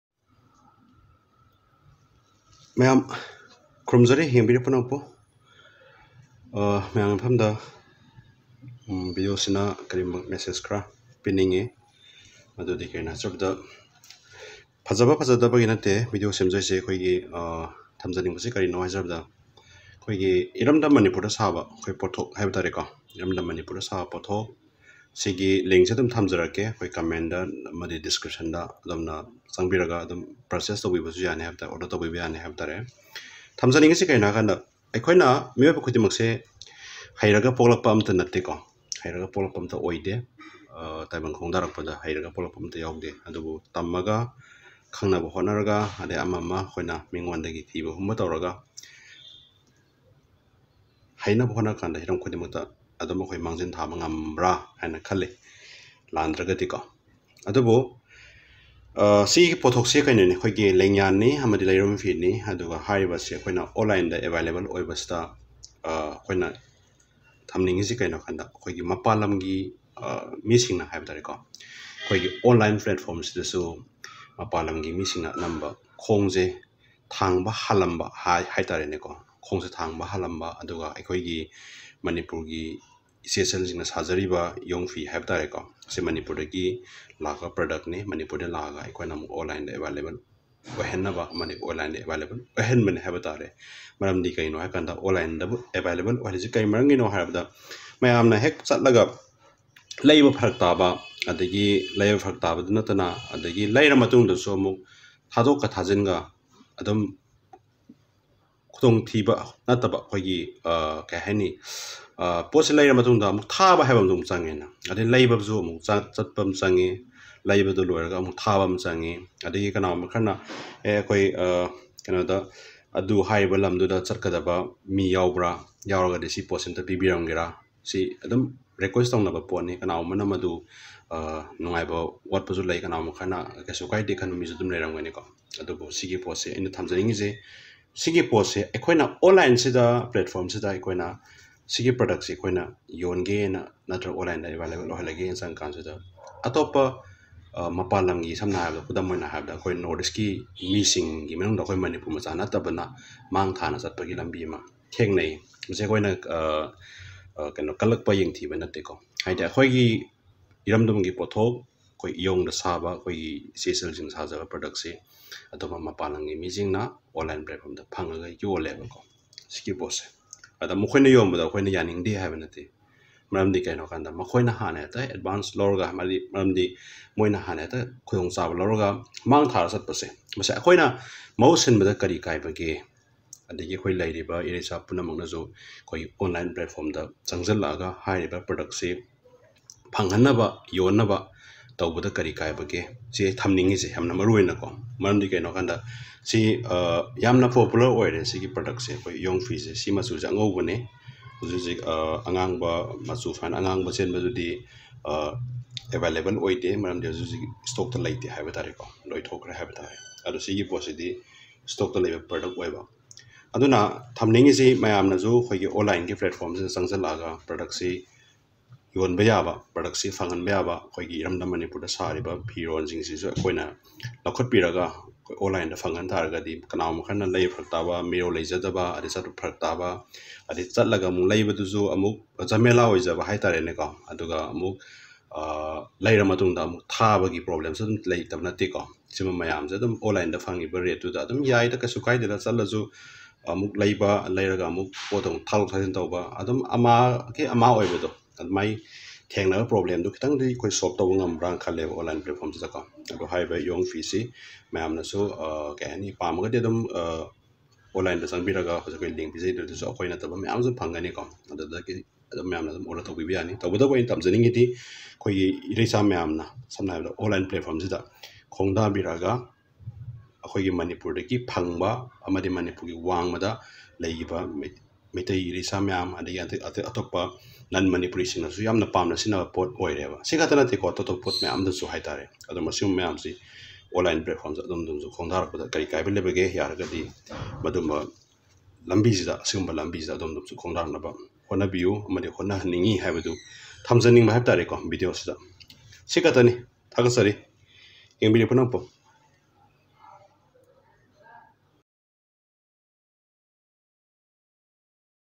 Eikhoigee Lengyan Phee.
Myam eikhoigi eramdamda phangba Lengyan phee ashi online da phanghanbada eikhoi eramdamgee yelhoumeesingna maru oina khumang chaosinna payhaningbagi apamba adu touhaningbagi wakhallonda video ashi semjabani.